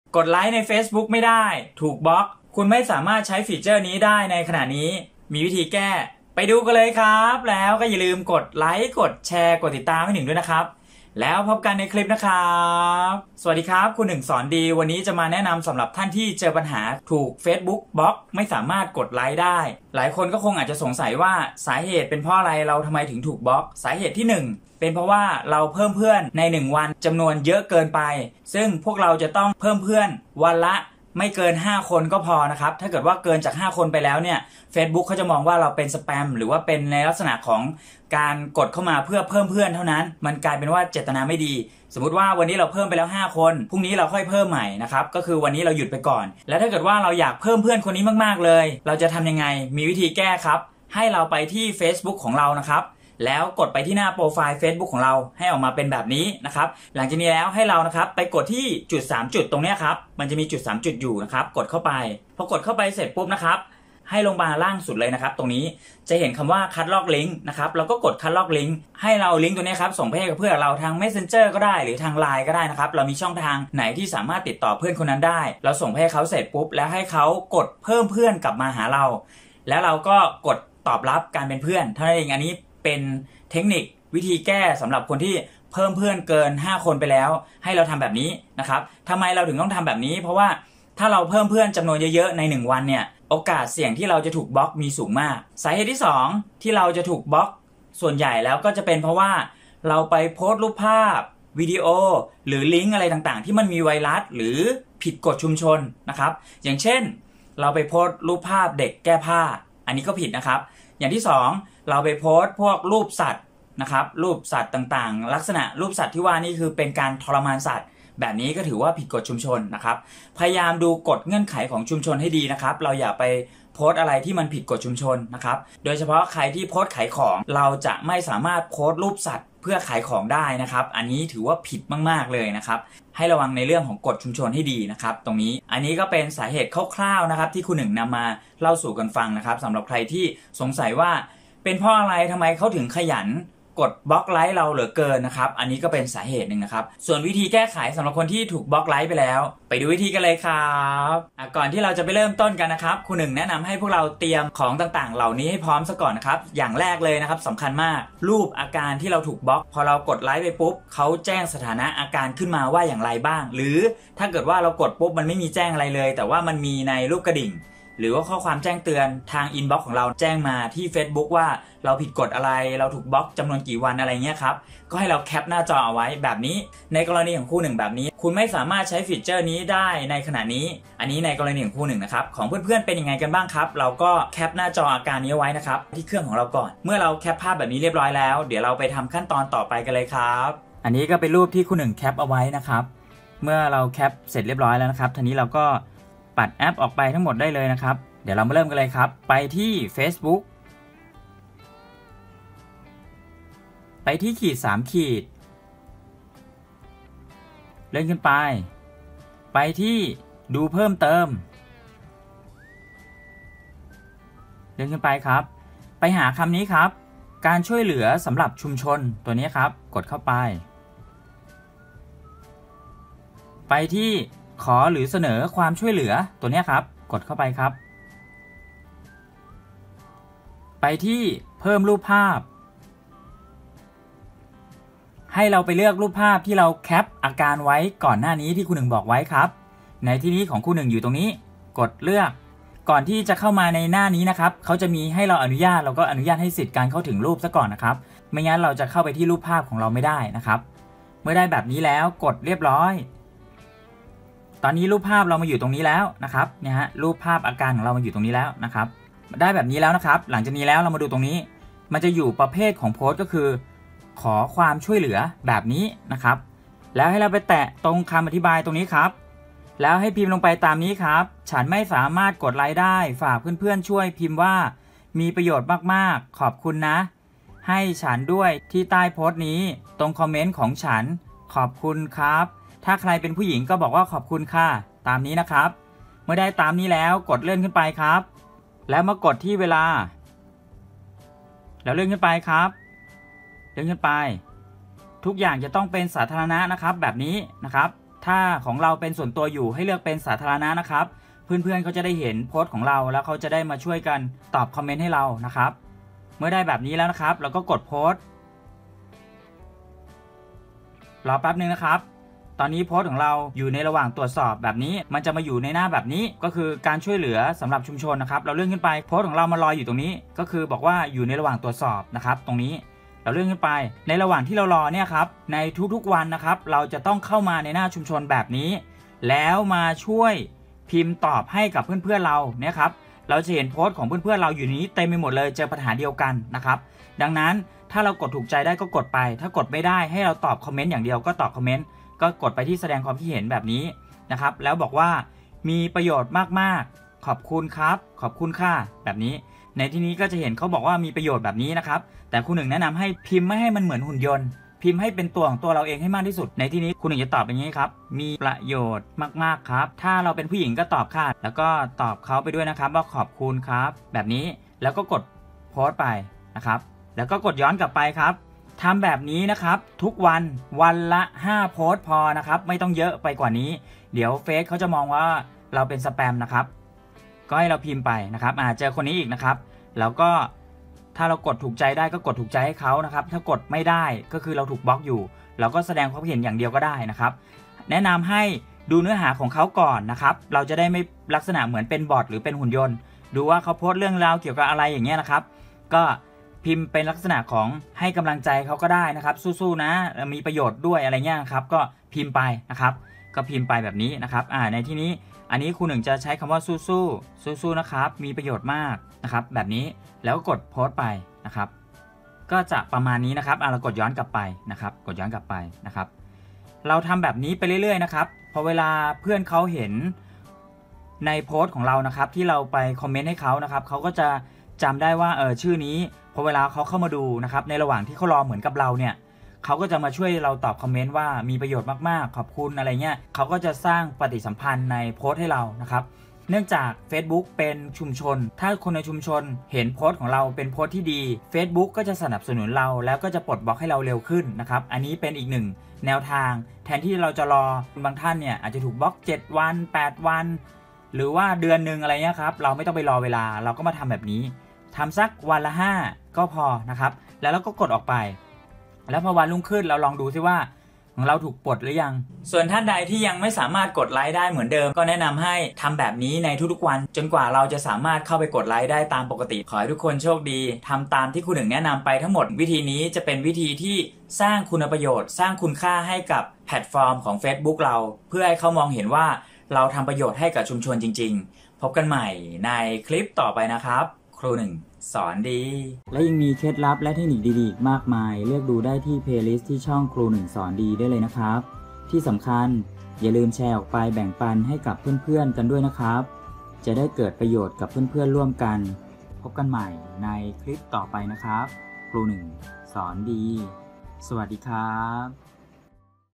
กดไลค์ไม่ได้ facebook เฟสบุ๊คถูกบล็อกชั่วคราว แก้เองได้ เห็นผลจริง ล่าสุด 2021 l ครูหนึ่งสอนดี
Facebook กด Like ไม่ได้ ให้ทำตามนี้ได้เลยนะครับ